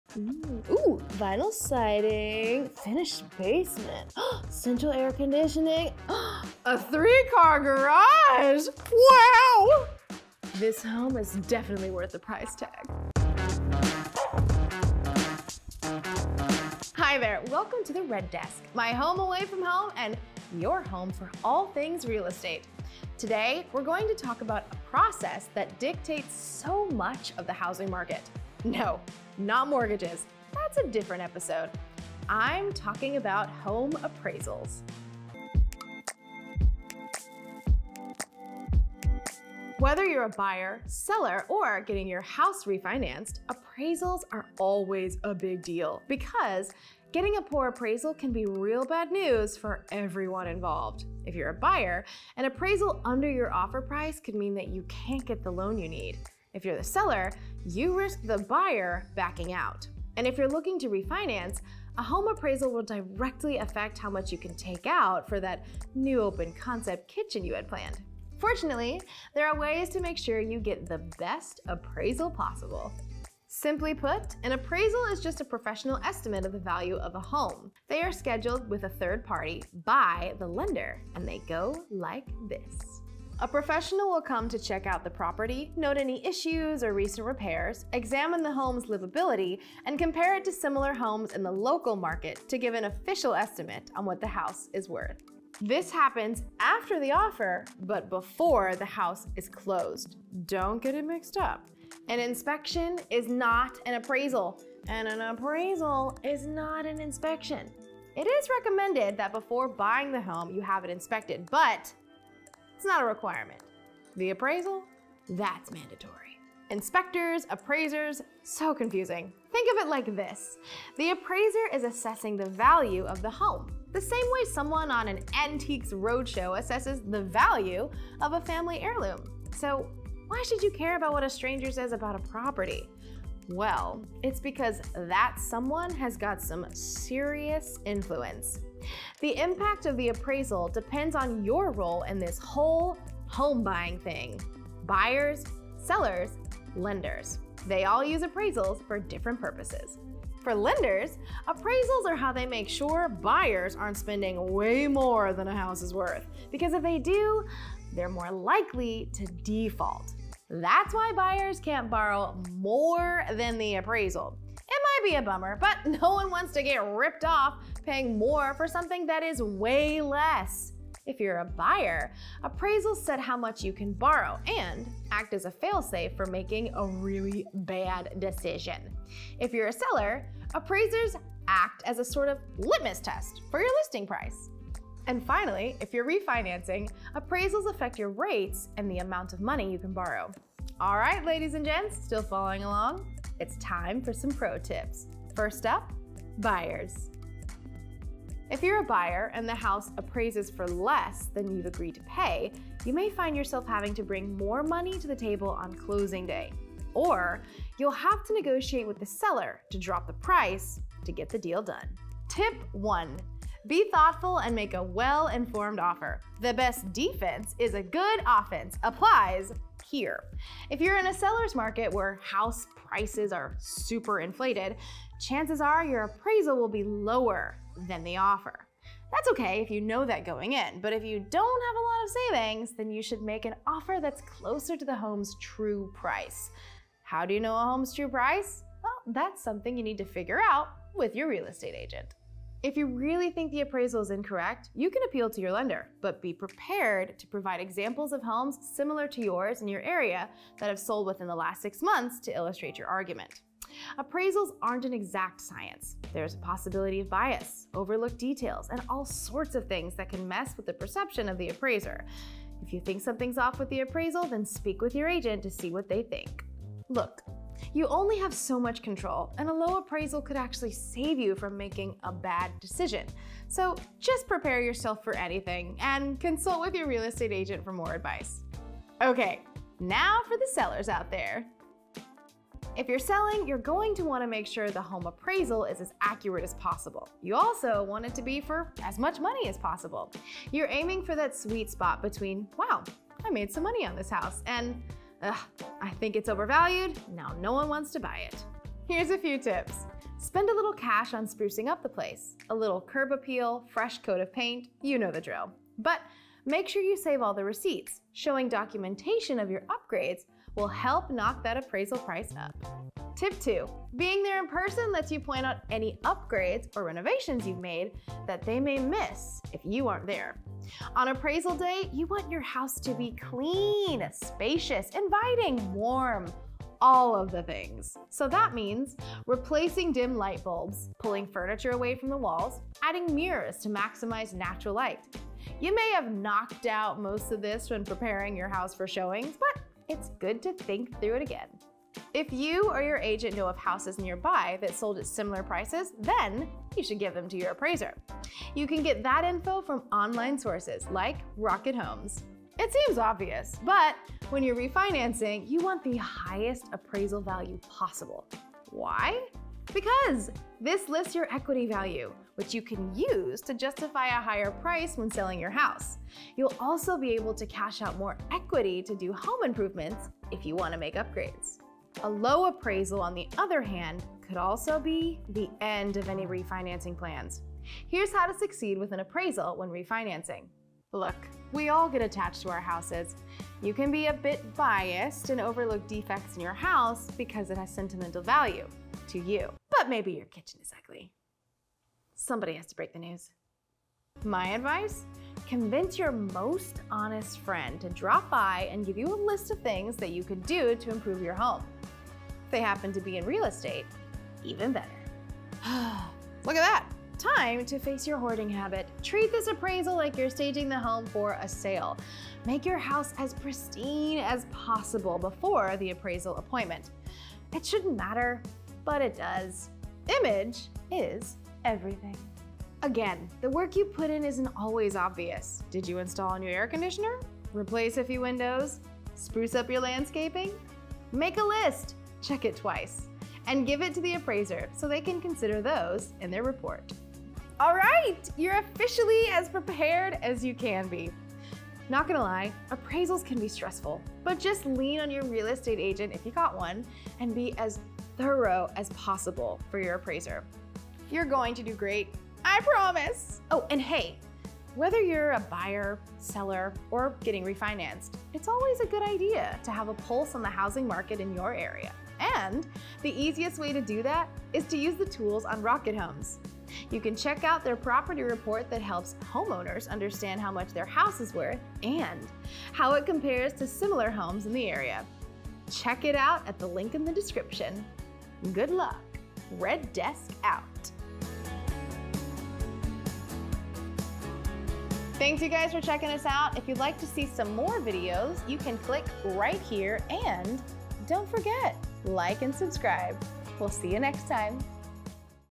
The Home Appraisal Process | The Red Desk
In this episode of The Red Desk, we’re going to break down the home appraisal process and how it affects both buyers and sellers.